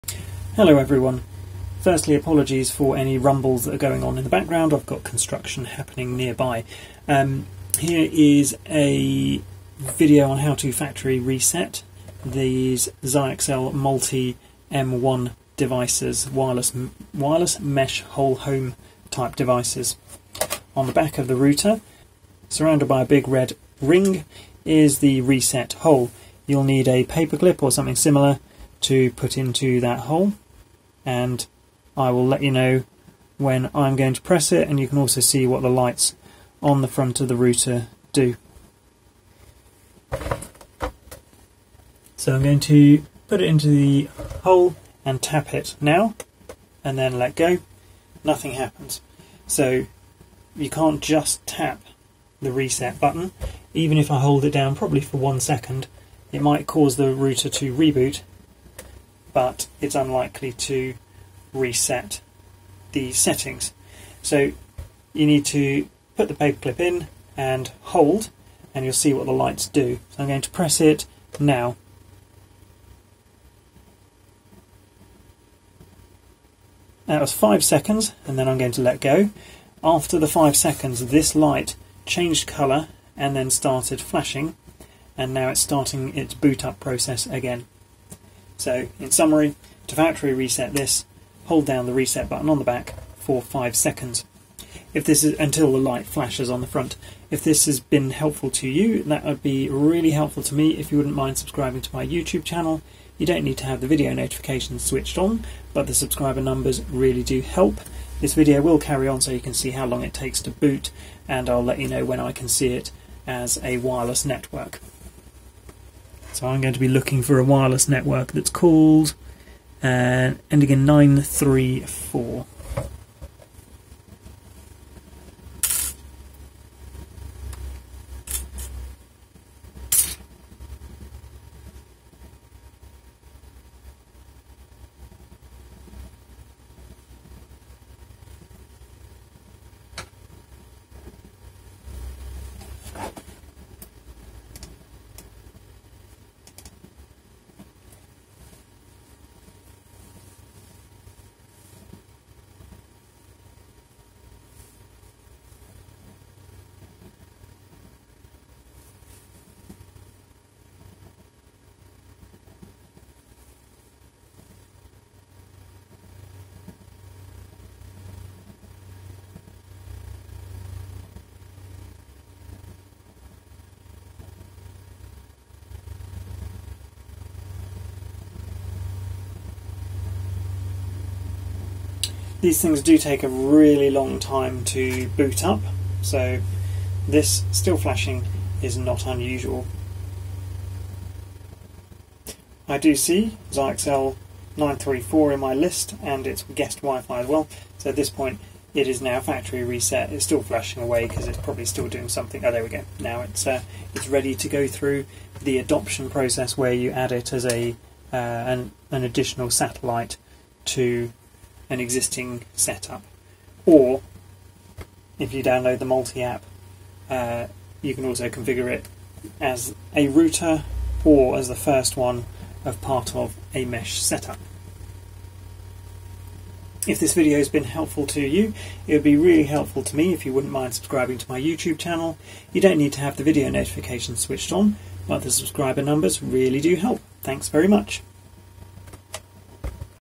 How to factory reset a Multy M1 WSM20
tl;dw: Hold down the reset button on the back for 5 seconds until the light on the front changes colour and starts flashing.
UPDATE: These devices are massively unstable and unreliable. I would like to say: Avoid where possible, buy something that isn't Zyxel for mesh.

How to factory reset a Multy M1 WSM20 wireless mesh node/device.